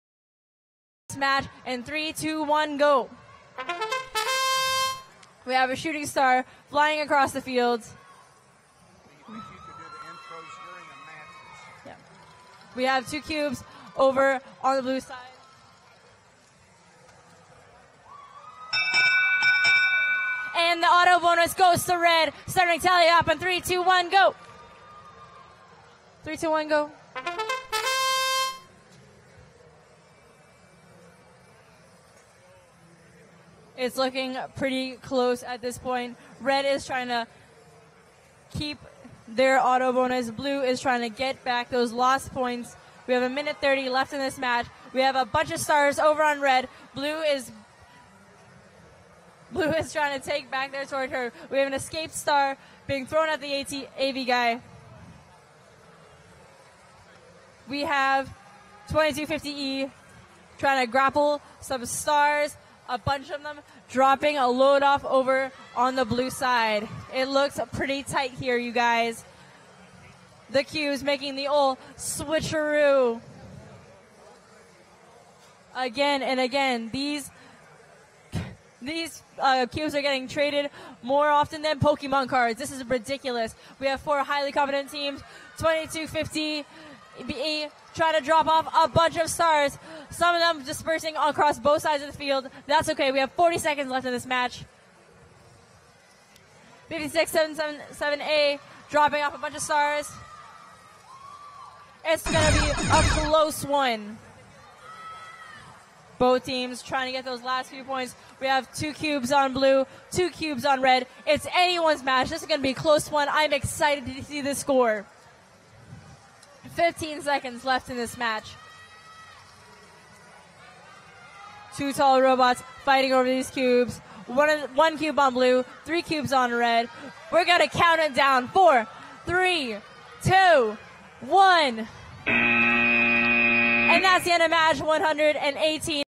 2017 VRC Res Q218 - 2250E 56777A vs 98725B 127C - 13 to 34
VEX Worlds 2017 - VRC High School - Research Division - Qualifiers Match 218

Red - Score 13:
2250E (Brigida Alvarez- HS ADV, Brigida Alvarez Rodriguez, Vega Baja, Puerto Rico)
||  Research Rank:58 . Wins:4 . Loses:6 . WP:8 . AP:28 . SP:143

56777A (Stargazer, Shenzhen High School of Science, Shenzhen, Guangdong, China)
||  Research Rank:77 . Wins:3 . Loses:7 . WP:6 . AP:16 . SP:116

Blue - Score 34:
98725B (Falcon, Rochester Hills Christian School, Rochester Hills, Michigan, United States)
||  Research Rank:31 . Wins:6 . Loses:4 . WP:12 . AP:20 . SP:152

127C (Lemon Bots, Campo Verde High School, Gilbert, Arizona, United States)
||  Research Rank:66 . Wins:4 . Loses:6 . WP:8 . AP:12 . SP:162